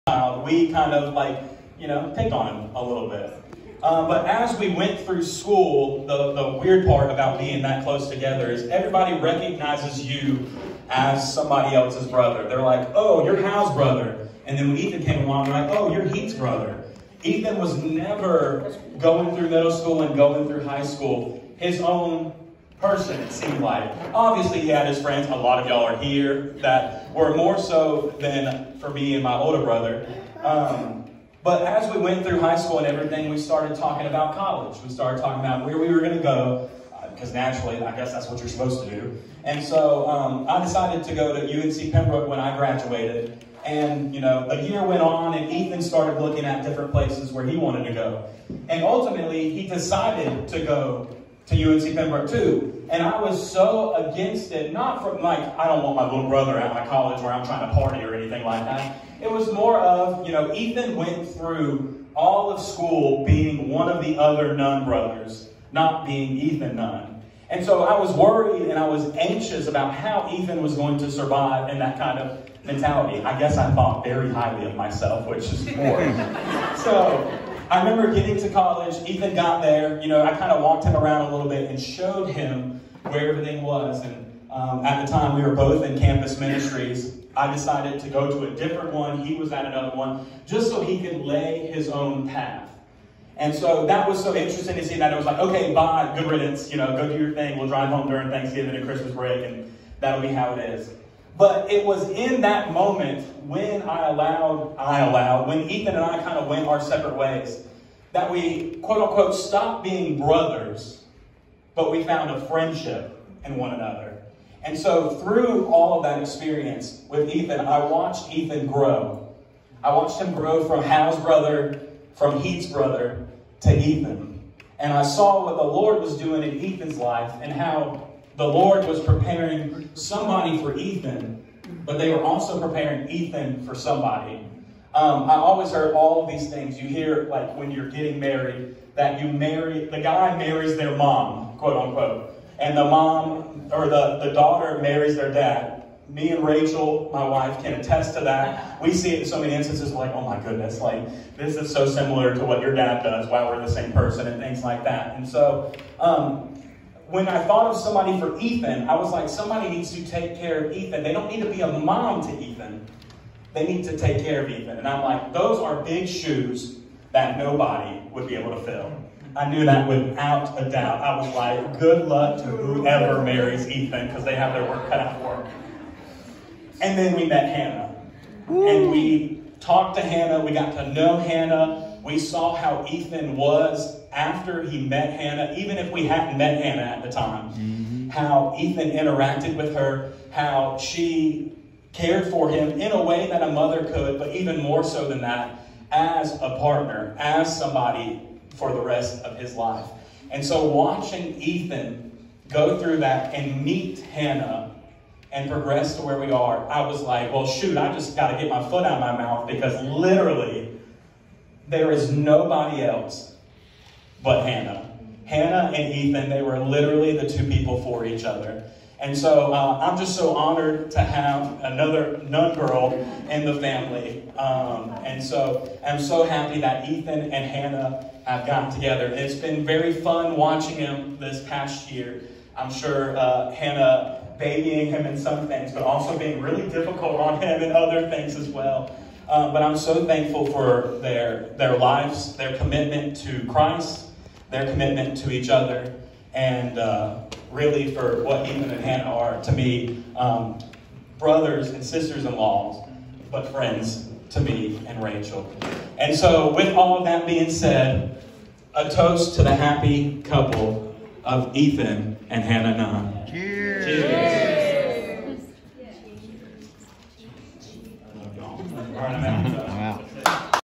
Heath “Best Man” Speech at Ethan and Hannah Wedding Reception
Heath Best Man Speech at Ethan and Hannah wedding reception at Starlight Meadow in Burlington NC